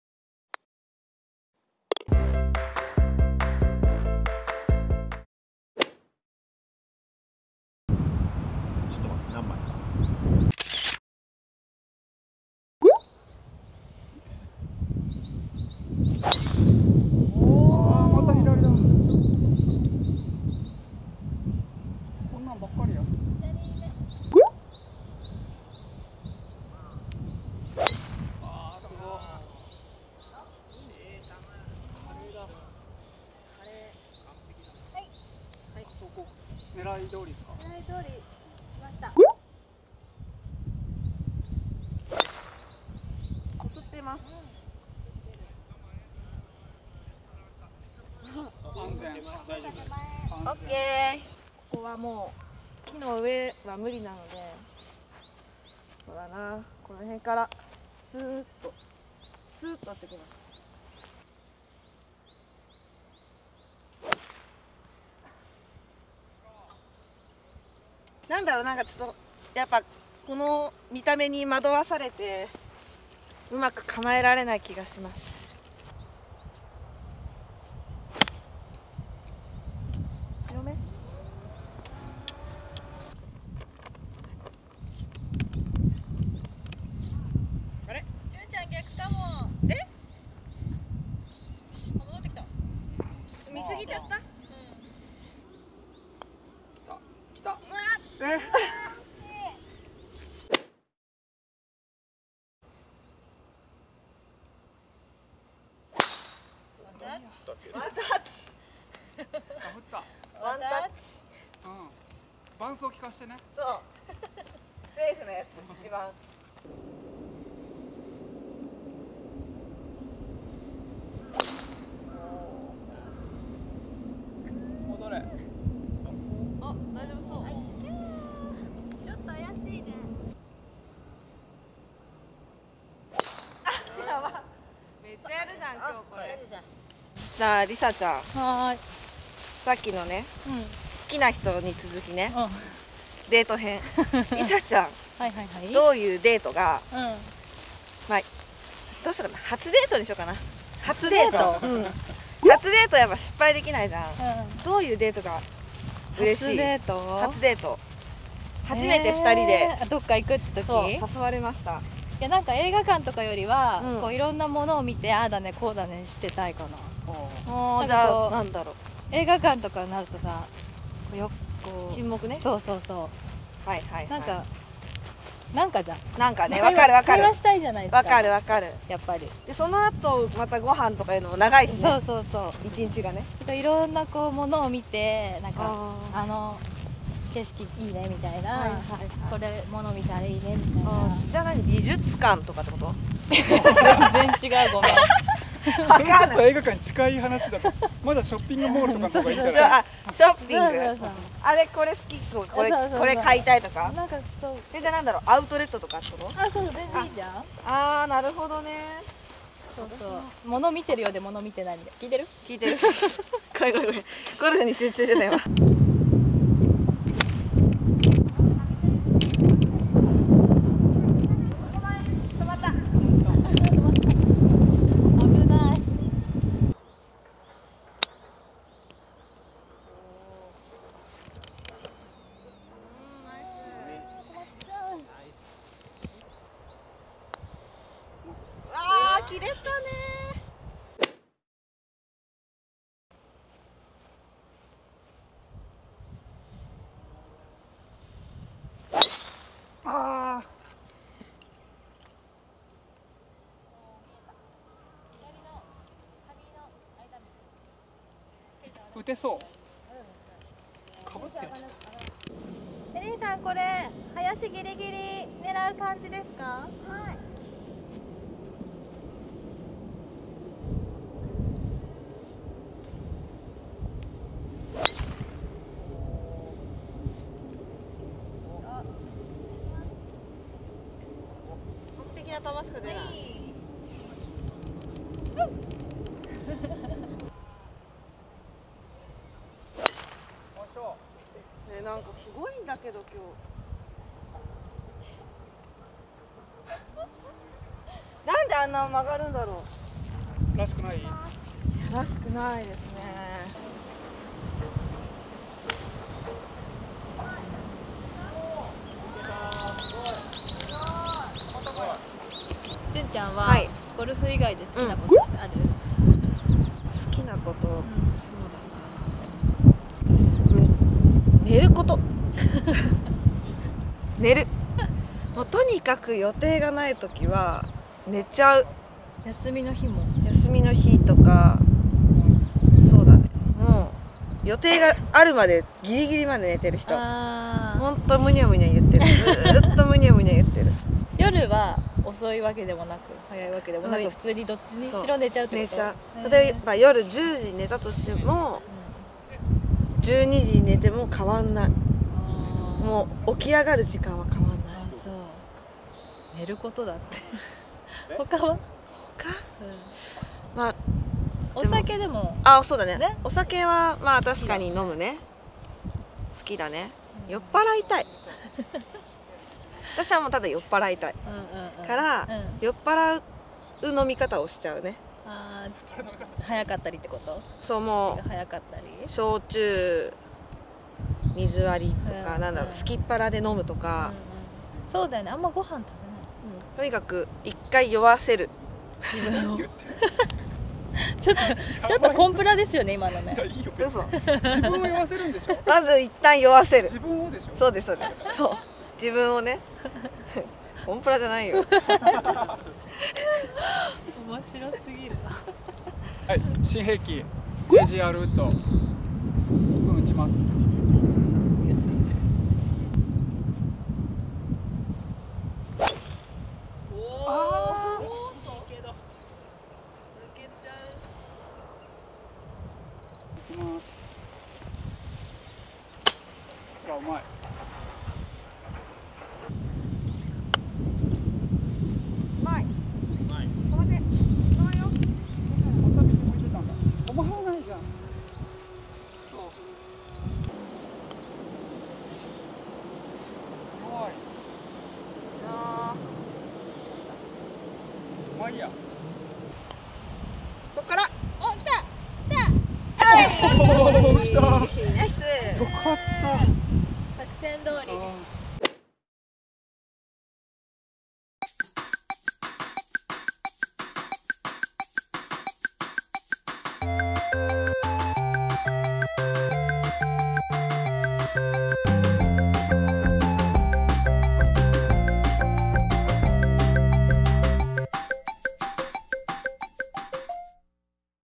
じゅんちゃんのお酒の飲み方を聞いたら・・ 【大利根カントリークラブ #4】
じゅんちゃんとりさちゃん、兄がWINWIN STYLEさんのコンペに参加してきました！
今回の目標は賞を取ること！
(兄は密かに100切りを目指してます)
じゅんちゃんとりさちゃんがお互いに質問するコーナーもあります！

♣本日のゴルフ場:大利根カントリークラブ

ーーーーー
アパレルここから予約できます↓

ーーーーー